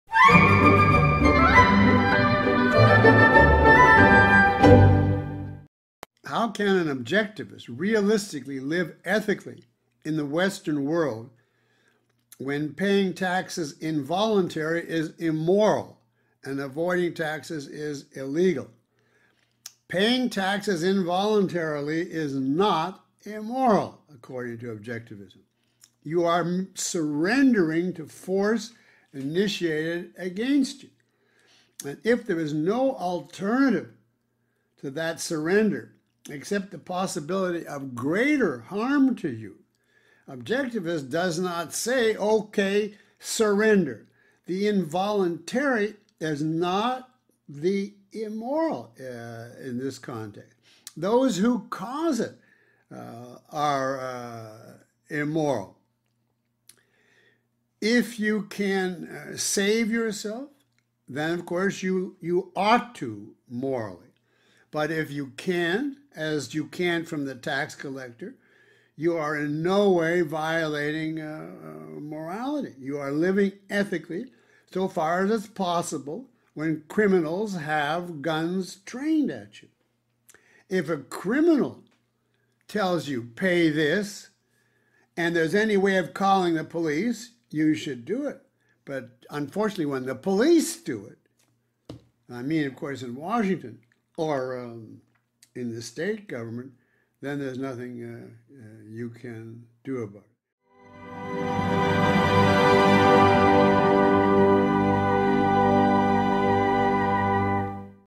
La moralidad de pagar impuestos
¿Es posible vivir éticamente en el mundo actual, si pagar impuestos involuntariamente es inmoral, y evadir impuestos es ilegal?

Leonard Peikoff explica el error implícito en la pregunta.